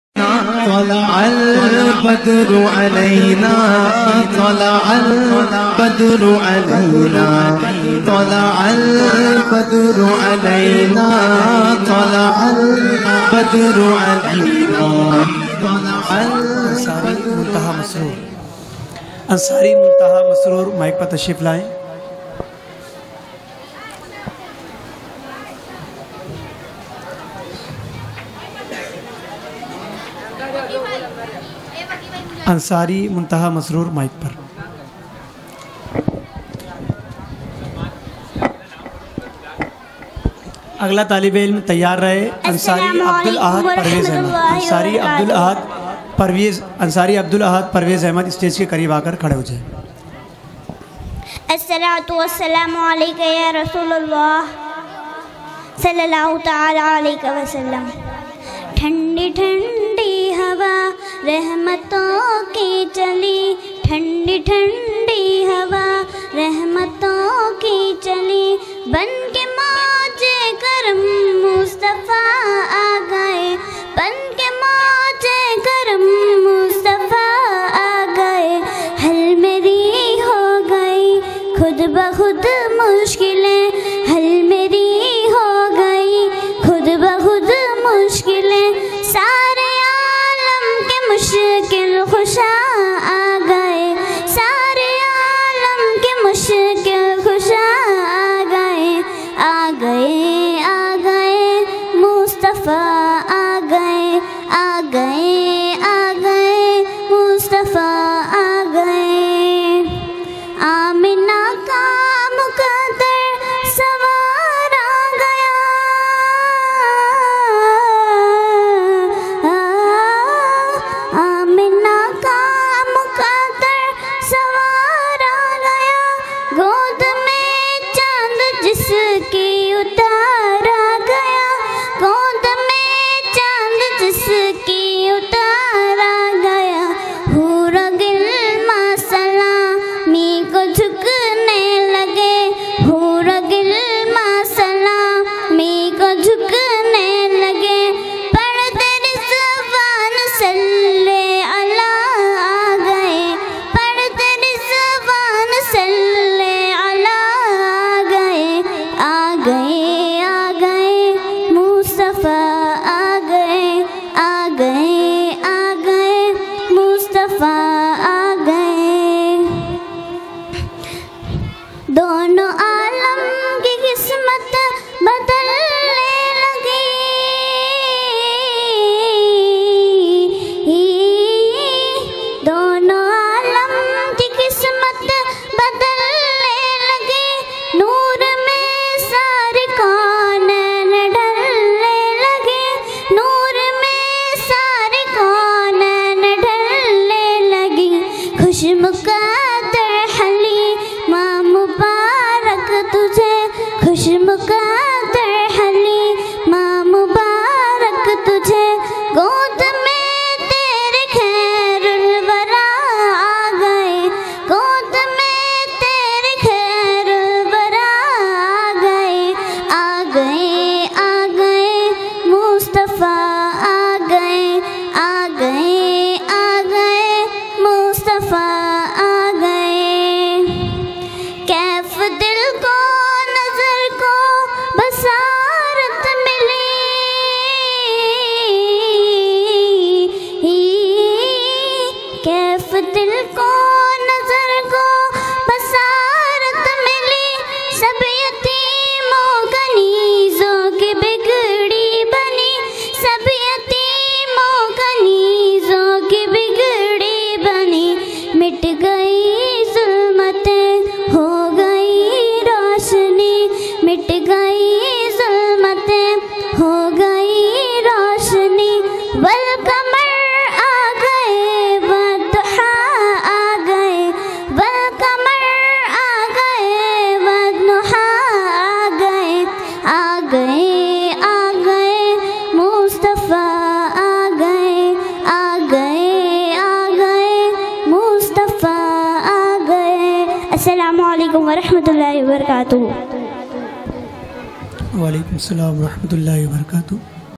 Ansari Muntaha Mashrur , Naatya Muqabala , Bazme Mohammadi Madrasa , Khajurpura Bhiwandi.
Ansari Muntaha Mashrur , ORGANIZED By Bazme Mohammadi Madrasa , Khajurpura Bhiwandi. On 1st Dec. 2018 ***,---VIDEO RECORDED & UPLOADED BY --"SKP MEDIA" , "SADIQUE K.P",... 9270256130,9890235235,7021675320 .